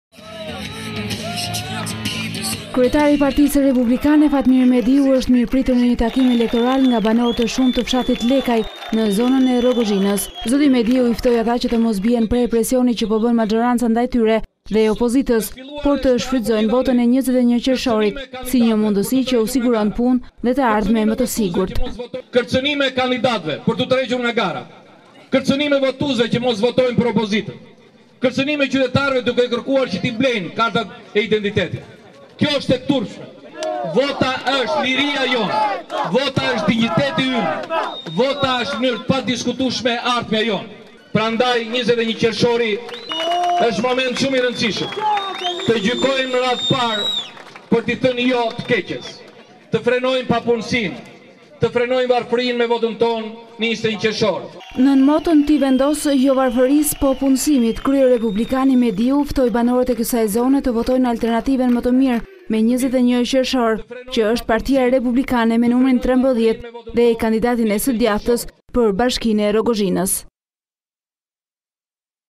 A1 Report - Mediu: Mos bini pre e presionit të qeverisë, votoni punësimin
Fatmir Mediu kryetar i Partisë Republikane i ka bërë thirrje qytetarëve që të mos bien pre e presionit që qeveria ushtron ndaj tyre dhe opozitës, thirrje të cilën e bëri nga fshati Lekaj në Rogozhinë.

Mediu në takim me banorët e këtij fshati ka ftuar qytetarët që të votojnë për alterantivën e punësimit si pjesë e programit të partisë së tyre tha gjithashtu se është koha ti thonë jo të keqes së instaluar prej dy vitesh.

Mediu i ftoi ata që të mos bien pre e presionit por të shfrytëzojnë votën e 21 qershorit e të votojnë PR me numrin 13 dhe kandidatin e të djathtës për Bashkinë e Rrogozhinës,si një mundësi që u siguron punë dhe të ardhme më të sigurtë nën moton "Ti vendos, Jo varfërisë, Po punësimit"